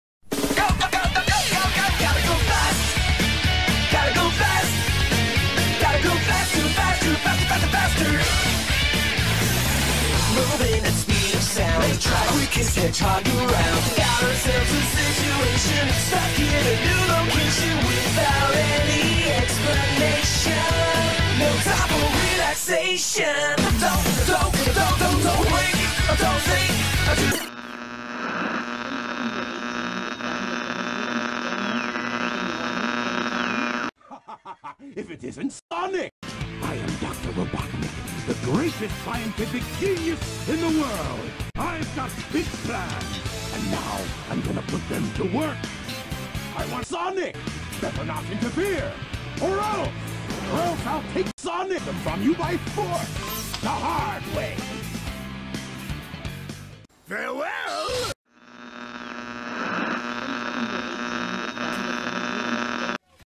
DR. EGGMAN HACKED MY CHANNEL!!!
Dr. Robotnik Hacks My Channel!

Guys! I was going to make a video for a cool annoucrment or a cool plush video but Dr. Eggman hacked the film like the Annoymus!
So the video couldn't roll.

On the comments below:
Roleplay on Dr. Eggman and John Summers!